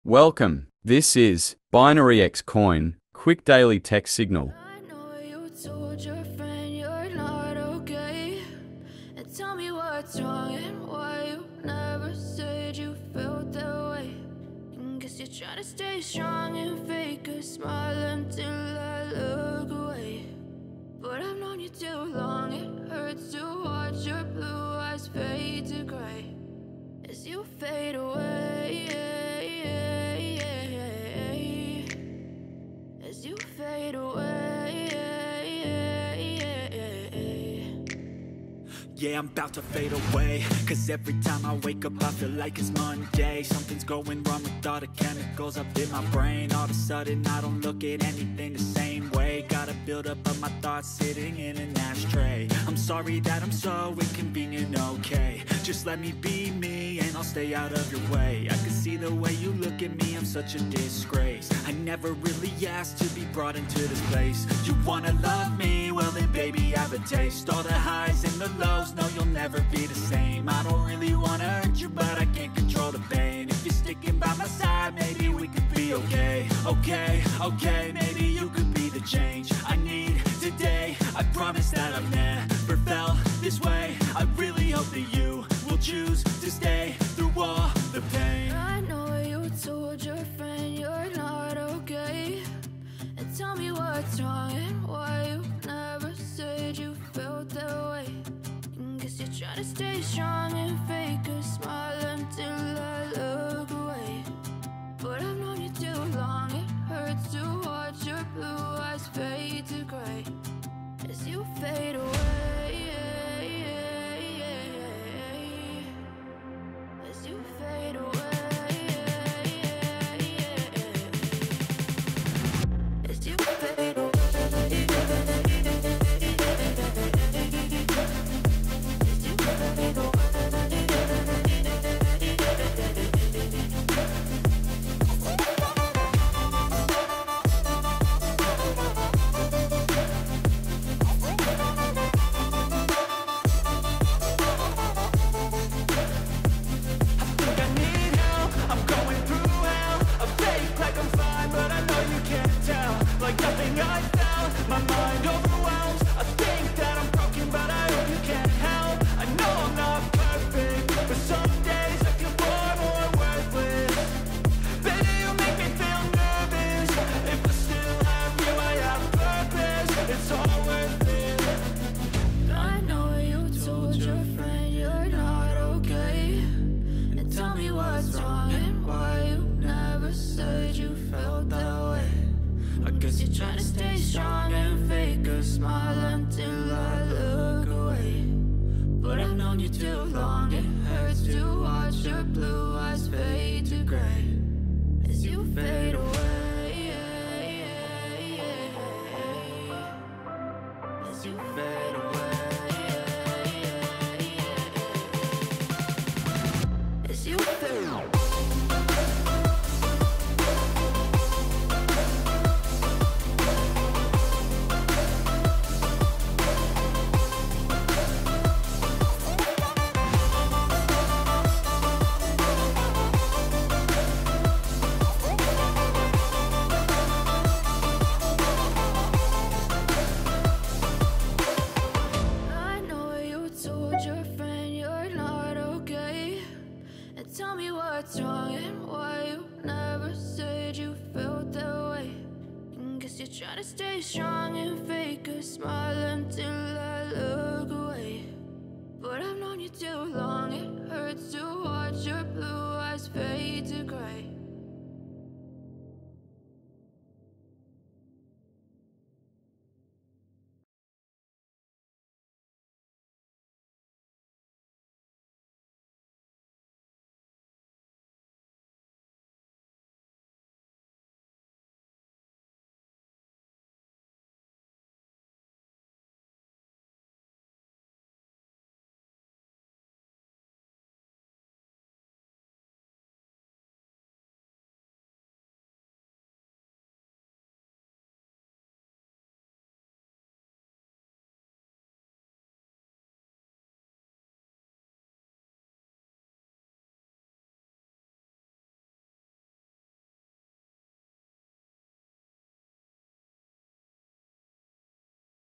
REVEALING BINARY X COIN TECHNICAL DAILY PRICE PREDICTION || BNX CRYPTO NEWS UPDATE LIVE!
In this video, I talk about BNX coin, BINARY X (OLD) Crypto.